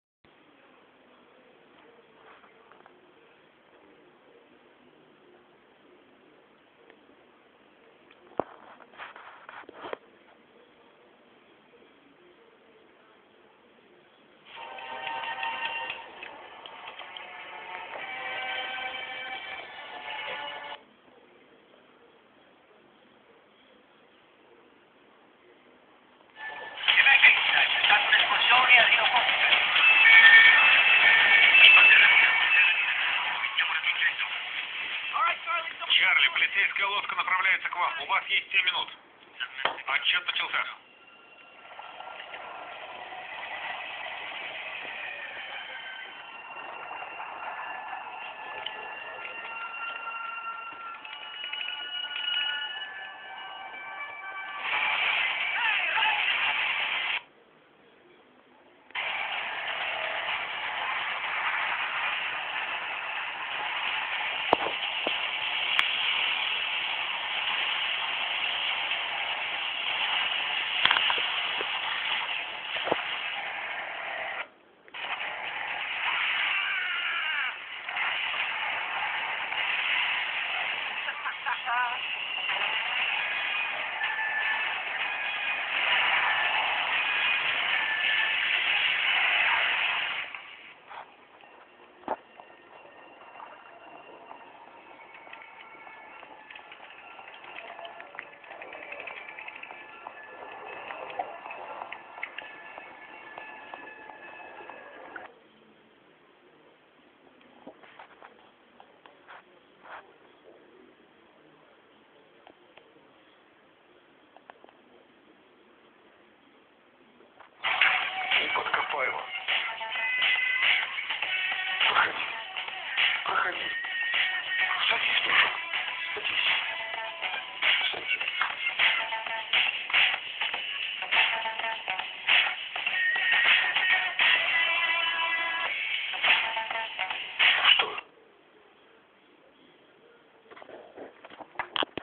Crystall TV на устройстве Symbian 9.2
программа интернет-телевидения Crystall TV через EDGE-соединение Киевстар (Украина)